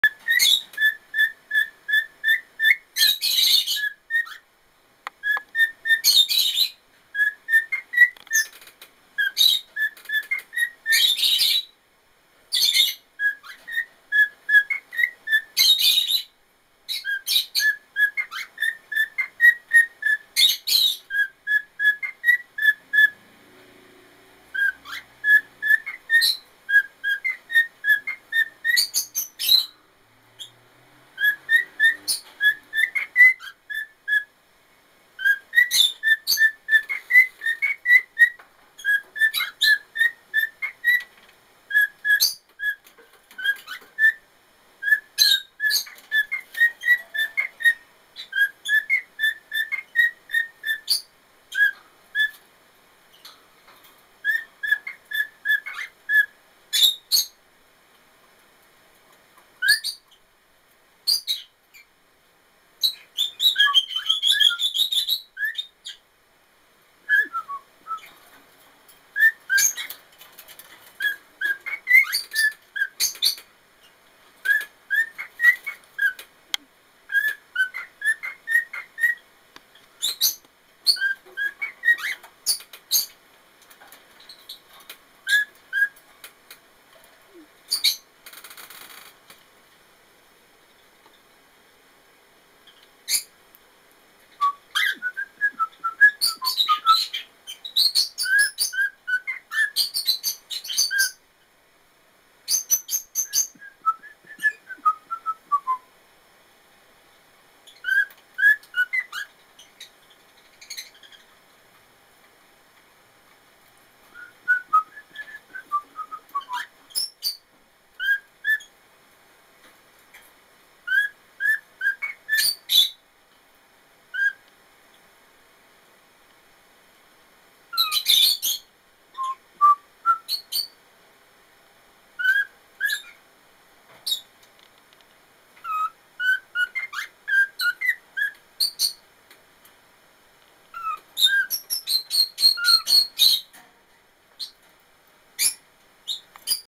Mysterious Song by Tokyo Cockatiel
Singing by Oh-chang@White-face Cockatiel 13yrs old male